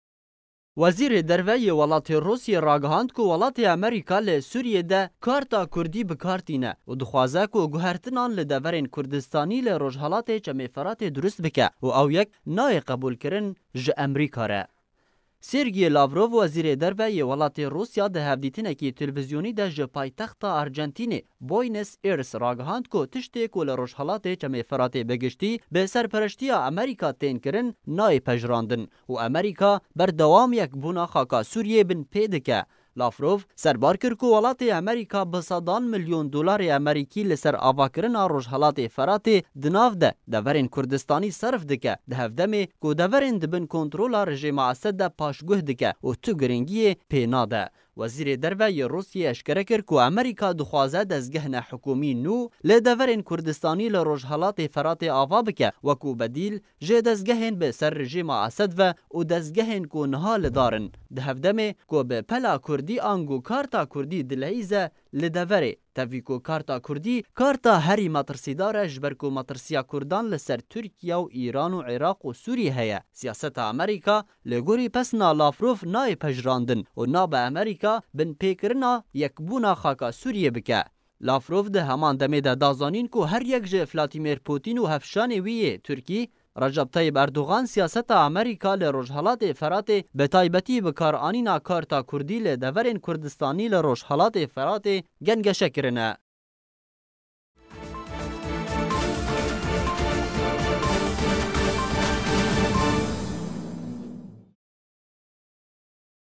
Lavrov: Amerîka Pela Kurdî bikartîne û ew leystok nayê pejrandin
لافروف: امريكا تلعب بالورقة الكوردية في سوريا

14.1